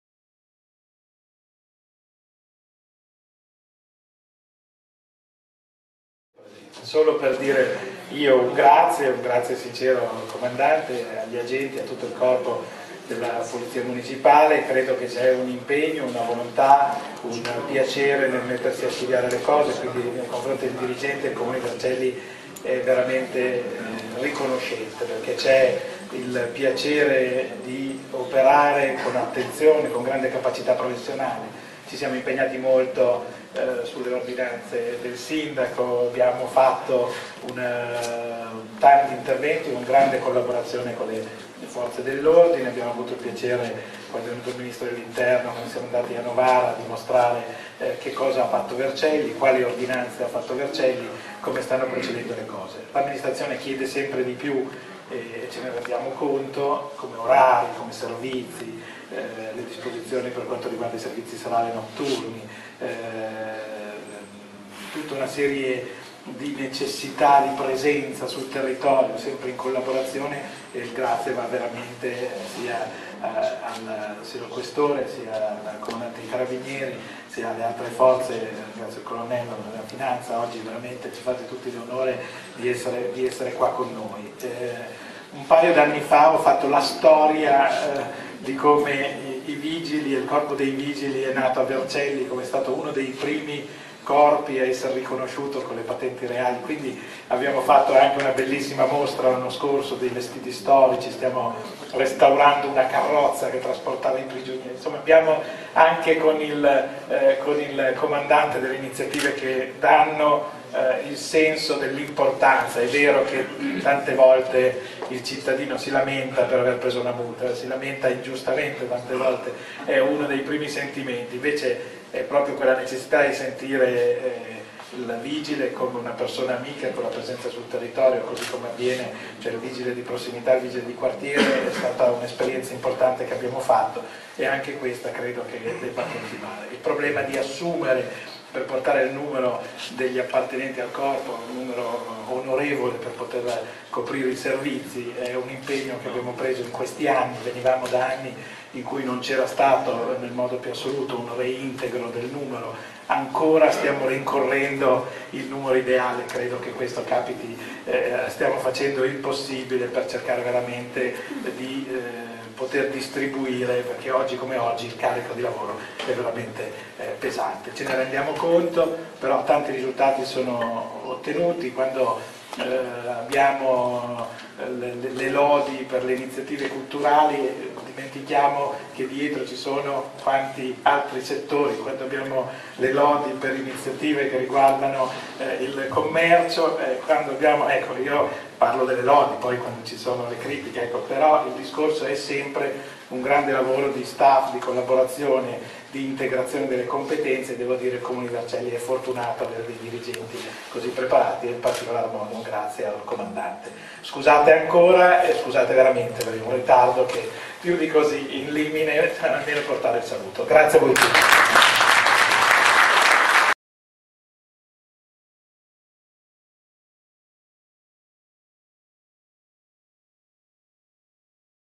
Festa della Polizia Municipale di Vercelli anno 2011 - parte PRIMA
Il saluto del Sindaco di Vercelli, Andrea Corsaro, in occasione della festa di San Sebastiano (Patrono del Corpo di Polizia Municipale)

Giovedì 20 gennaio 2011
(Video: Giorgio Morera)